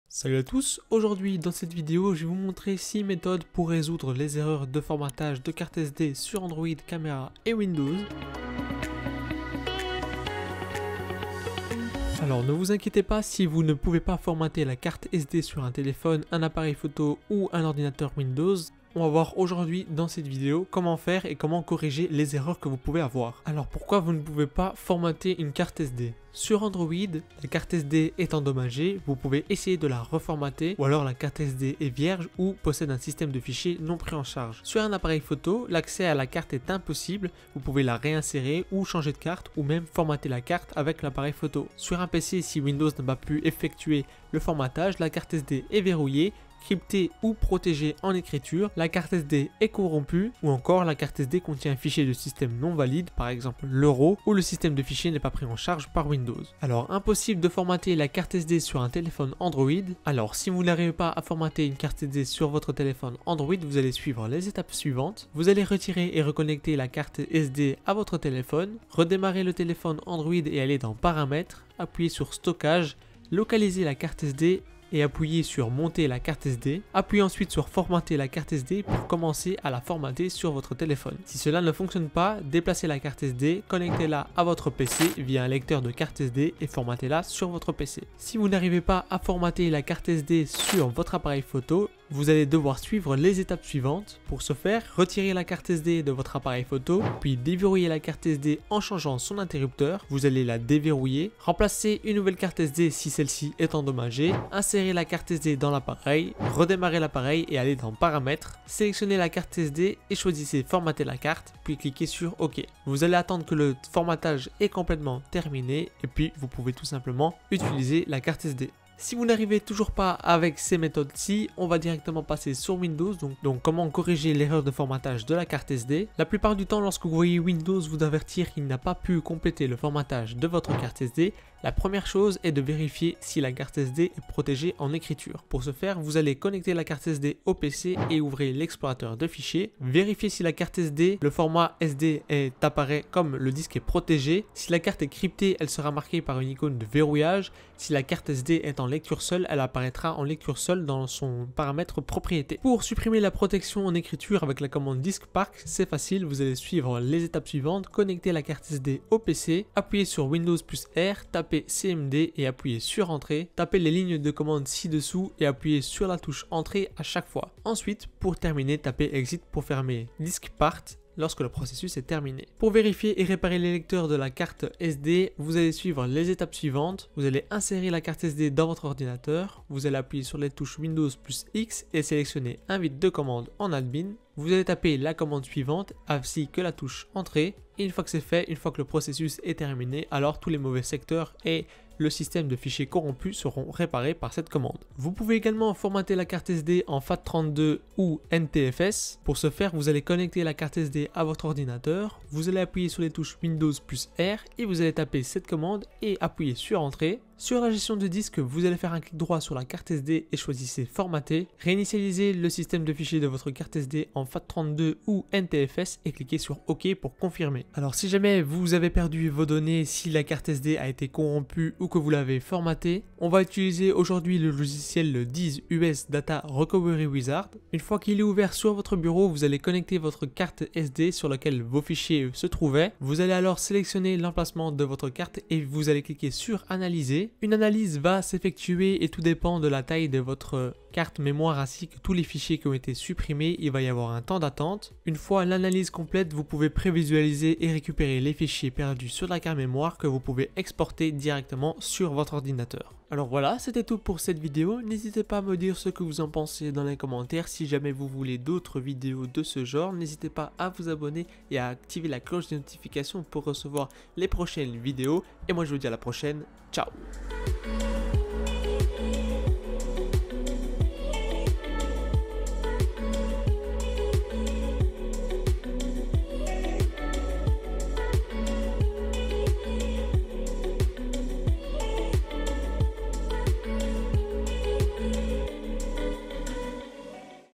Impossible de formater la carte SD sur PC Windows ou Android [6 Méthodes]
🔗Télécharger EaseUS Data Recovery Wizard Free pour récupérer données carte SD

Cette vidéo présente pourquoi carte sd impossible à formater et comment formater une carte SD qui refuse le formatage ? Essayez les méthodes pour corriger l’erreur « Impossible de formater la carte SD» sur les téléphones Android, les appareils photo et les PC Windows.

📌Timestamp
00:13 Pourquoi je ne peux pas formater ma carte SD ?
01:04 Partie 1. Impossible de formater la carte SD sur Android
01:35 Partie 2. Corriger l'erreur de formatage de carte de SD sur Canon
02:02 Partie 3. Corriger l'erreur de formatage de carte de SD sous Windows
03:46 Récupérer des données perdues sur une carte SD formatée

Guide étape par étape pour corriger l’erreur « Impossible de formater la carte SD»

▬▬▬▬▬▬▬▬▬▬▬ VIDÉOS ASSOCIÉES  ▬▬▬▬▬▬▬▬▬▬▬

impossible de formater carte micro sd protégé en écriture
formatage carte sd impossible windows 10